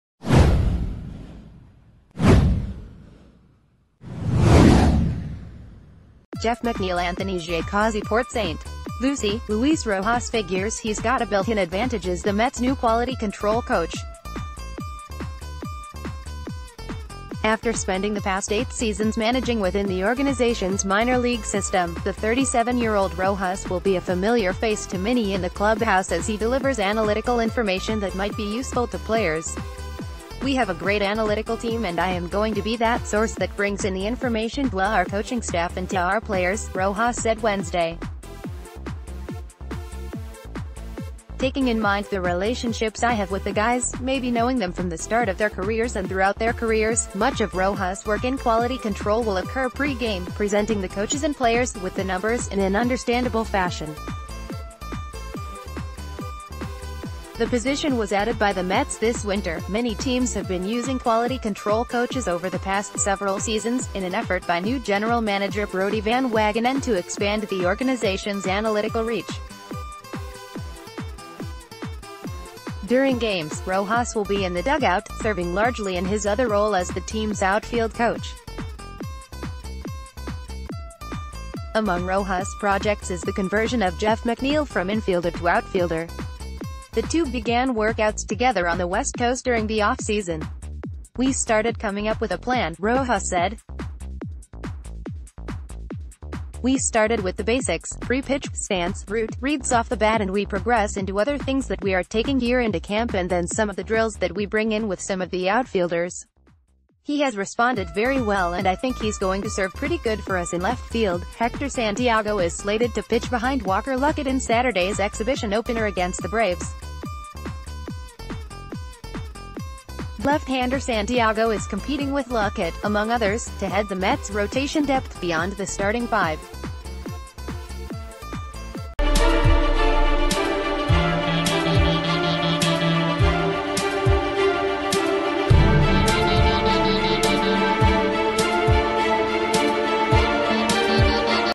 Mets’ new coach working on Jeff McNeil’s outfielder conversion
Jeff McNeil Anthony J. Causi PORT ST. LUCIE — Luis Rojas figures he’s got a built-in advantage as the Mets’ new quality control coach. After spending the past eight seasons managing within the organization’s minor league system, the 37-year-old Rojas will be a familiar face to many in the clubhouse as he delivers analytical information that might be useful to players. “We have a great analytical team and I am going to be that source that brings in the information to our coaching staff and to our...